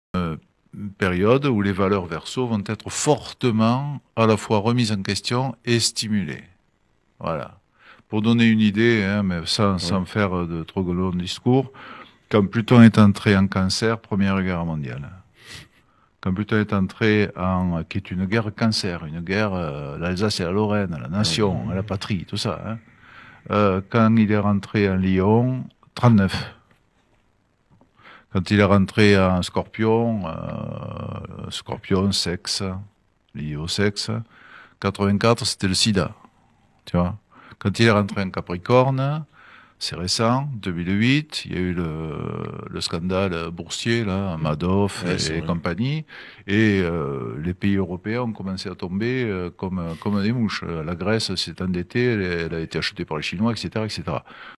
Décryptage astrologique : Sylvain Pech vous dévoile les grandes tendances de 2024 !
Que va t'il se passer en 2024 ? Comprendre l'astrologie avec Sylvain Pech, astrologue ☀️
Pour voir l'émission en entier RDV sur notre chaîne Youtube, lien en bio